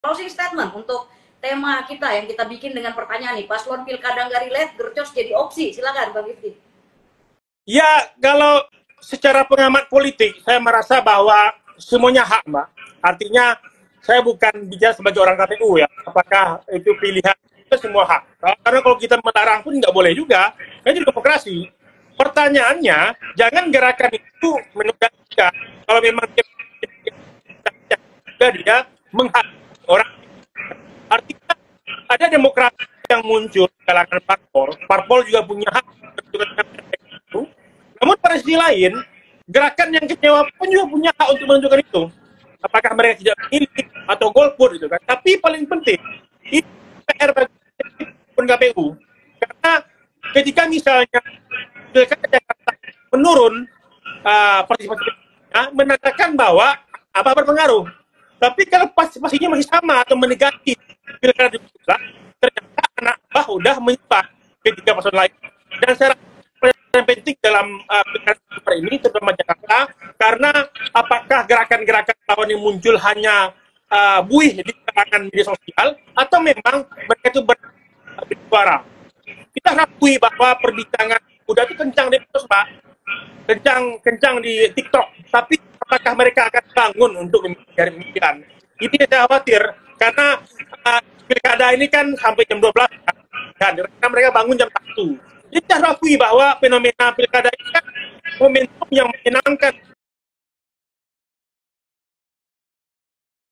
PERGERAKAN POLITIKAL MENYIKAPI HAK BERPENDAPAT DI ERA DIGITAL
Pergerakan atau aksi-aksi yang tidak relevan dengan substansi pilkada. Jika kita melarangnya, kita juga melanggar hak demokratis mereka. Setiap gerakan, baik yang mendukung atau yang kecewa, memiliki hak untuk menunjukkan pendapat mereka, entah itu partai politik atau individu.

"Masalah lain dan sekarang yang penting dalam aplikasi ini terutama Jakarta karena apakah gerakan gerakan tawon yang muncul hanya Buih dikatakan media sosial. kita akui bahwa perbincangan kuda itu kencang di TikTok," Direktur Eksekutif Aljabar Strategic/ Pengamat Politik, Arifiki Chaniago.